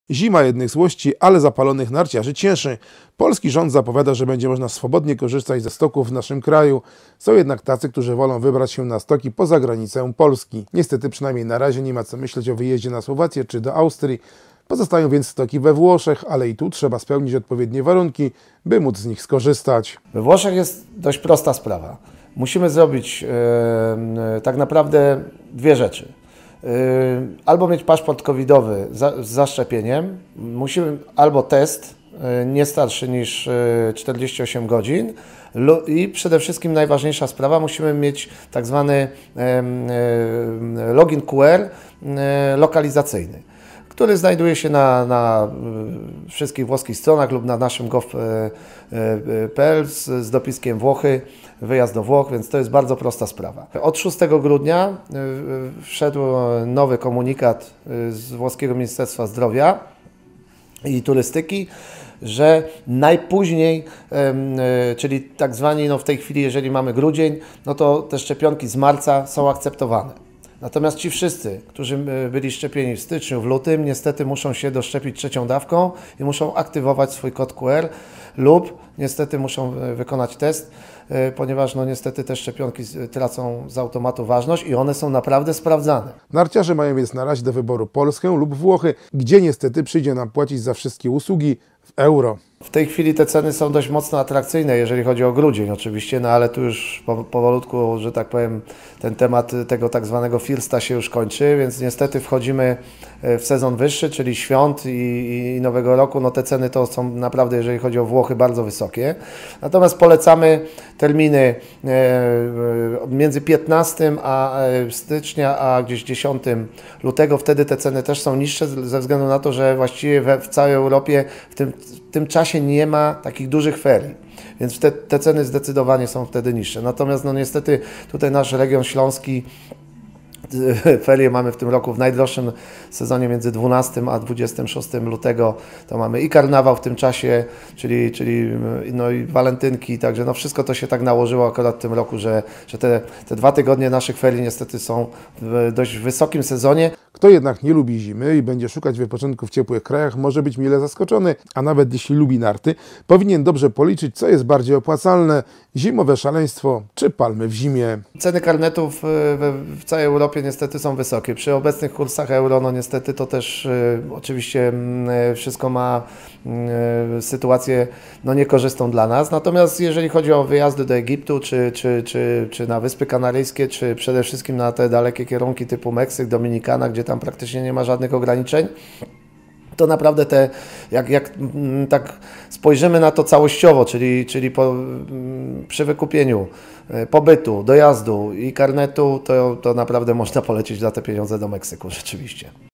Meksyk tańszy niż narty?
Zimowy wypoczynek w Meksyku tańszy  niż zimowe szaleństwo? O tym w naszym materiale.